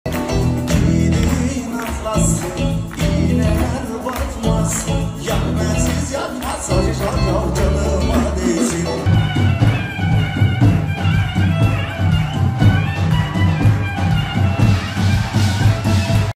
Demet Evgar'dan sahne şovu... Hem söyledi hem dans etti
Demet Evgar'dan sahne şovu... Hem söyledi hem dans etti... Taner Ölmez ile düet yaptığı ve eğlendiği anlar izleyicilerin beğenisini topladı